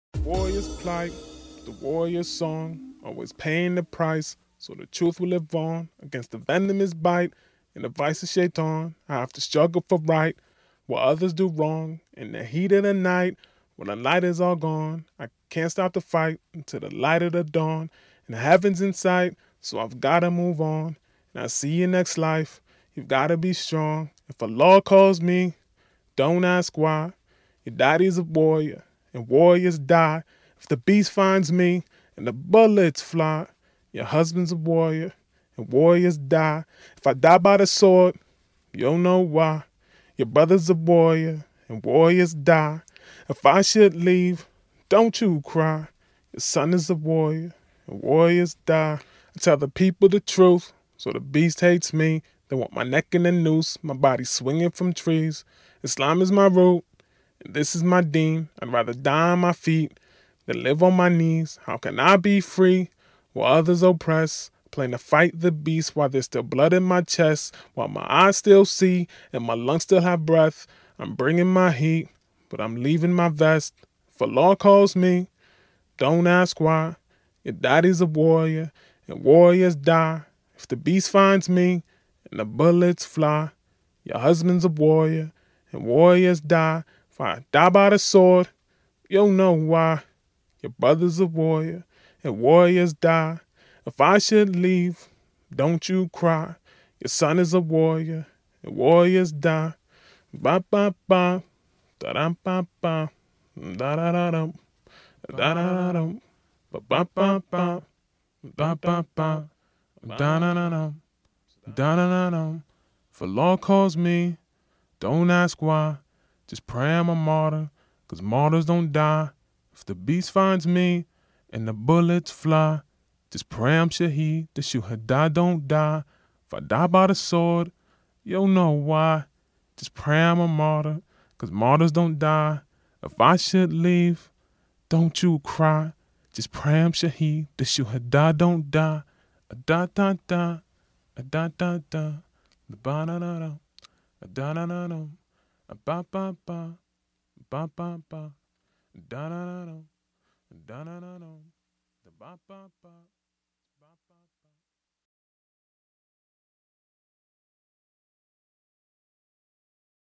Imam Haron mural
A graffiti mural commissioned in 2005 on the side of the Distrix cafe in District 6, Cape Town for the Imam and I documentary
The mural took 3 days and was made by world famous graf artists Faith 47 and Mak 1.